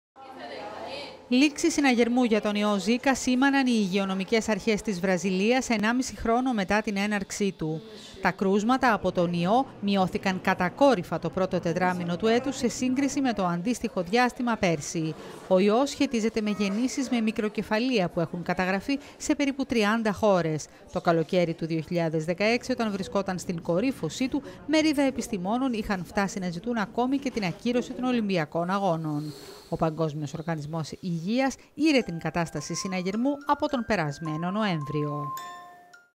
Βραζιλία: Λήξη συναγερμού για τον ιό Ζίκα
Λήξη συναγερμού για τον ιό Ζίκα σήμαναν οι "υγειονομικές αρχές της Βραζιλίας" ενάμιση χρόνο μετά την έναρξή του.

Τα κρούσματα από τον ιό έπεσαν κατακόρυφα (μείωση 95% των κρουσμάτων) το πρώτο τετράμηνο του έτους σε σύγκριση με το αντίστοιχο διάστημα πέρσι.

Ο ιός σχετίζεται με γεννήσεις με μικροκεφαλία που έχουν καταγραφεί σε περίπου 30 χώρες. Το καλοκαίρι του 2016 όταν βρισκόταν στην κορύφωσ…